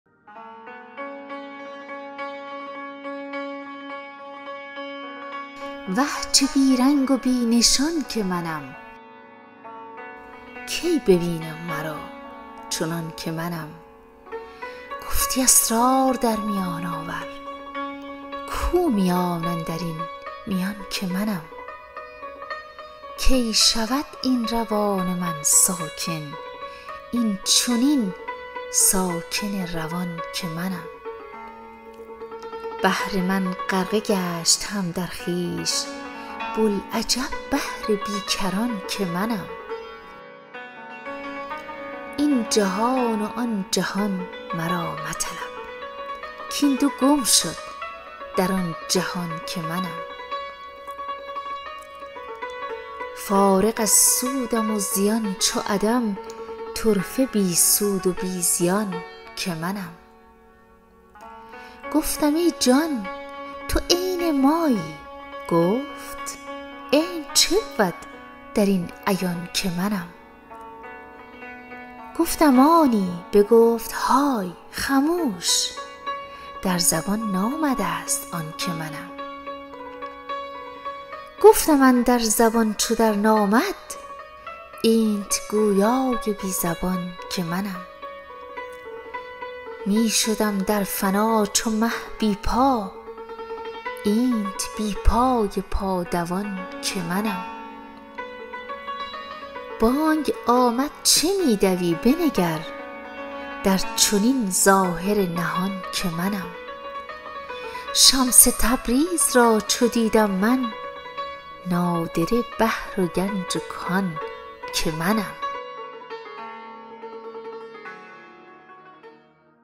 وه چه بی‌رنگ و بی‌نشان كه منم/ غزل عمیق مولانا#Молана#Шеъри форсӣ#sufism
این غزل شگفت مولانا.با مطلع زیبا، با پارادکس های زیبا مانند « ساکن روان »،« بی پای پا دوان »،« ظاهر نهان» ،« گویای بی زبان » شعری است پویا و تاثیرگزاربا ساختاری شاعرانه و جذاب
مرحله ای که انسان همه چیز را رها می کند و اصلا برای او مهم نیست که چگونه او را قضاوت می کنند یا دیوانه اش می خوانند. از خود بی خود شدن و رسیدن به کشف و شهودهای عارفانه،
در کل غزل شاعر با بازی های شاعرانه و استادانه مفهوم عمیق عرفانی شعر را رپر و بال می دهد.
در برخی نسخه ها ابتدای غزل با «اه» شروع شده که نادرست است؛ زیرا این صوت مناسب تنفر و خشم است و اسم صوت «وه»برای شگفت زدگی و شور مولانا مناسب است.
وه چه بی‌رنگ و بی‌نشان كه منم                  كی ببینم مرا چنان كه منم
گفتی اسرار در میان آور                       كو میان اندر این میان كه منم
كی شود این روان من ساكن                     این چنین ساكن روان كه منم
بحر من غرقه گشت هم در خویش            بوالعجب بحر بی‌كران كه منم
این جهان و آن جهان مرا مطلب           كاین دو گم شد در آن جهان كه منم
فارغ از سودم و زیان چو عدم               طرفه بی‌سود و بی‌زیان كه منم
گفتم ای جان تو عین مایی گفت            عین چه بود در این عیان كه منم
گفتم آنی بگفت‌های خموش                   در زبان نامده‌ست آن كه منم
گفتم اندر زبان چو درنامد                    اینت گویای بی‌زبان كه منم
می شدم در فنا چو مه بی‌پا                  اینت بی‌پای پادوان كه منم
بانگ آمد چه می دوی بنگر                  در چنین ظاهر نهان كه منم
شمس تبریز را چو دیدم من                 نادره بحر و گنج و كان كه منم

Recited by drzeynabnowruzi wisequotes# Poem rumiquotes rumi poetry#
Persian [English subtitle]Молана Шеъри форсӣ#
Mystic Mysticism love sufism#